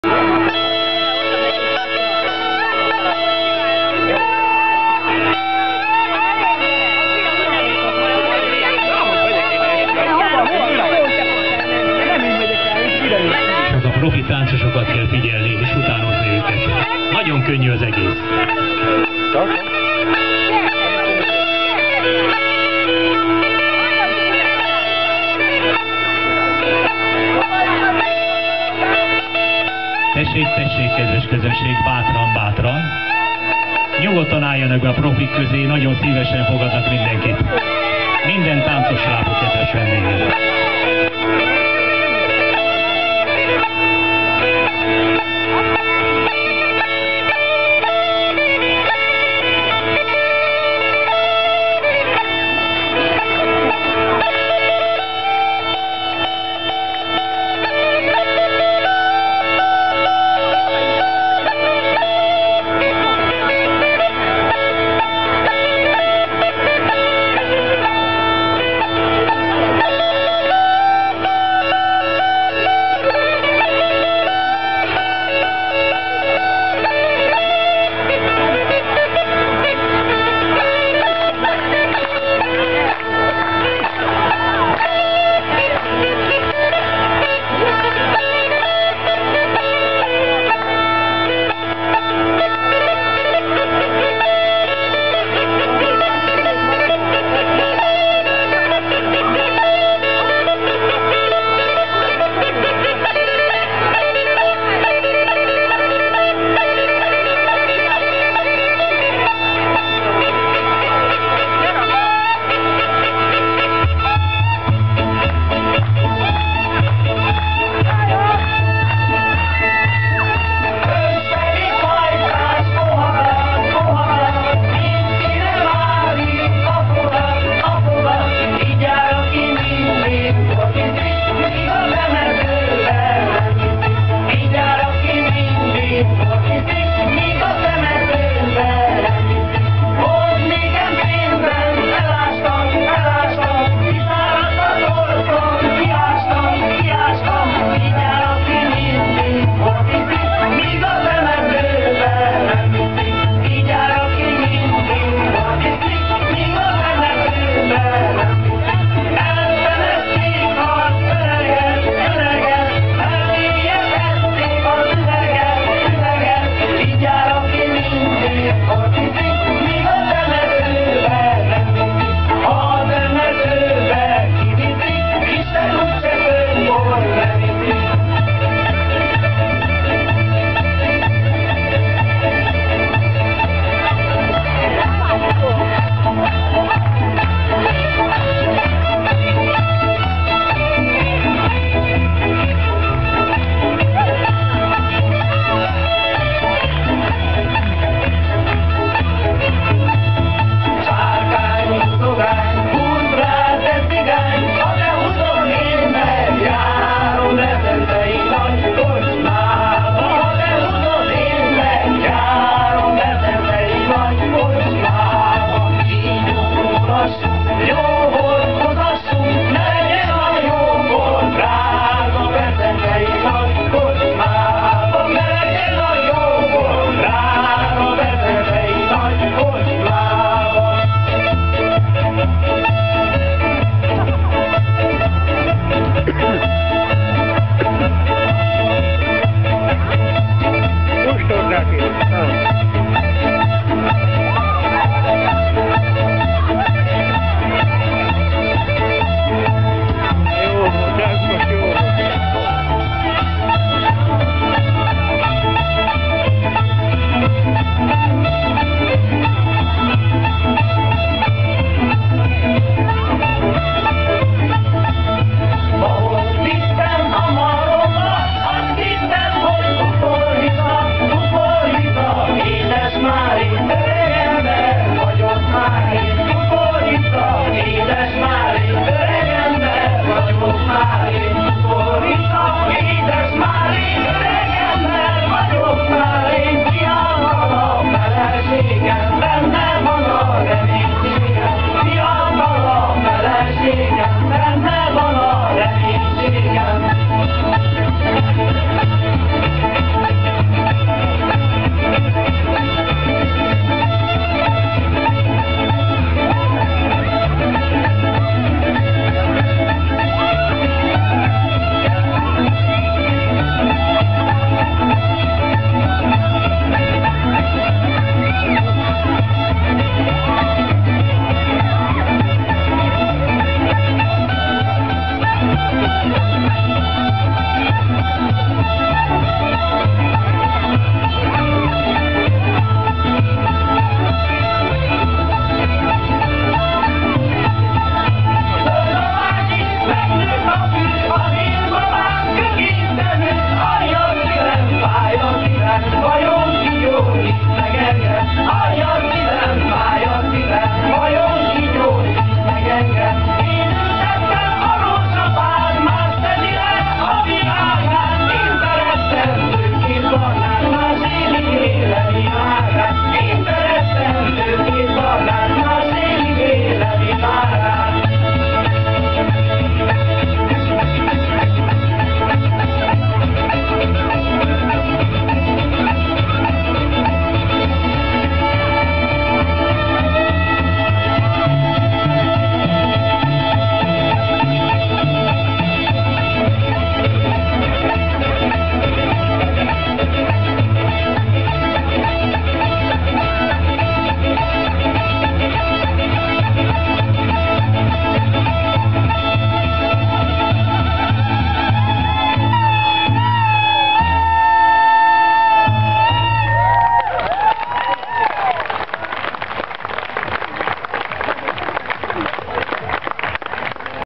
XIII. BAKONYI BETYÁRNAPOK. ORSZÁGOS BETYÁRTALÁLKOZÓ. 2011. AUGUSZTUS 12 -- 13. ZIRC 10.
Össztánc a színpad előtt